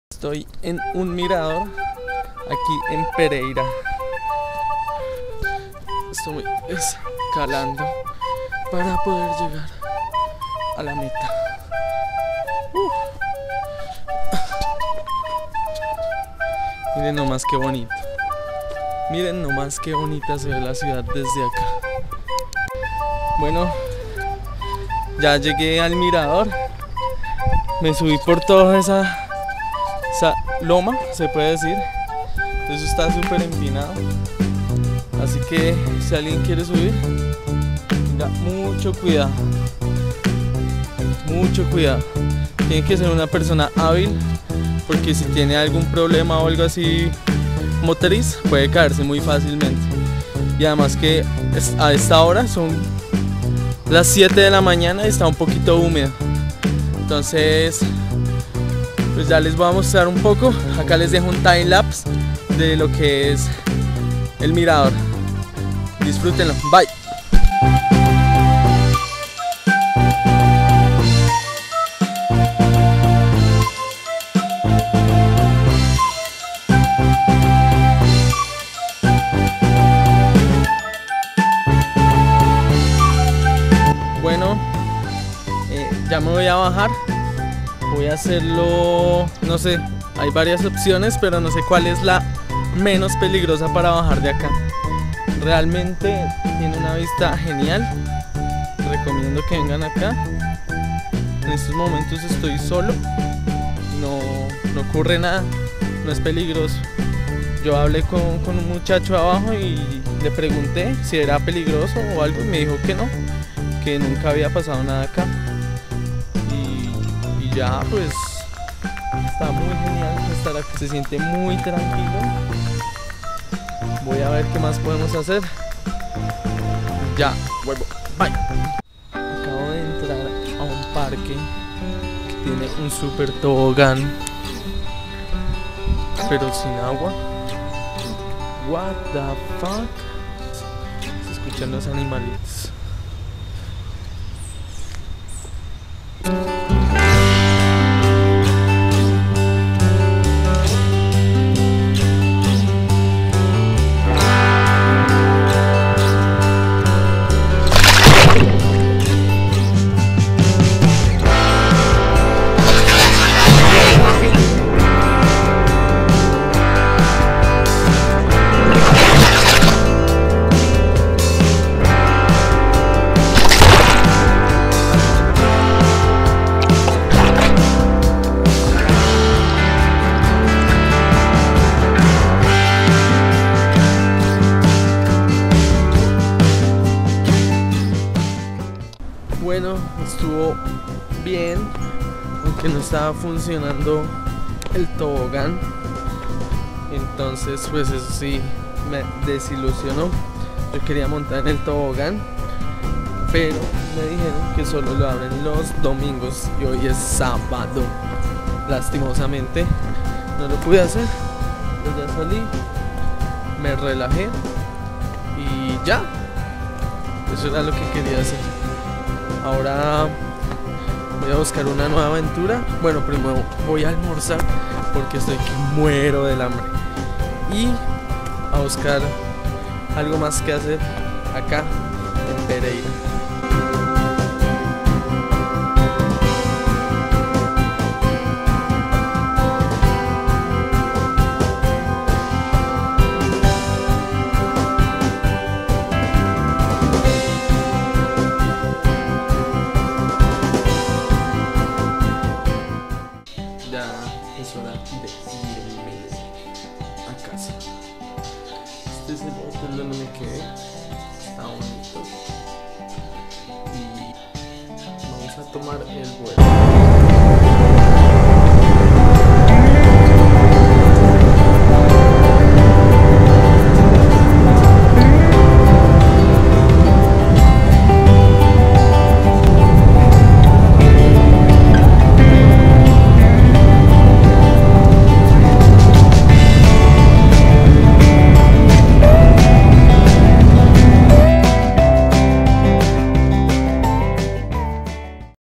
DESDE LO MÁS ALTO DEL MIRADOR - Pequeño Viaje en Pereira
Hola, hice este vídeo en mi viaje a Pereira, Colombia, es corto, pero quería intentar hacer un vídeo así, para poder recordar que algún día fui allí.

No se si alguien verá este vídeo, pero creo que no importa mucho, solamente quería hacer un vídeo de este tipo hace desde hace mucho tiempo, así sea solamente para mi.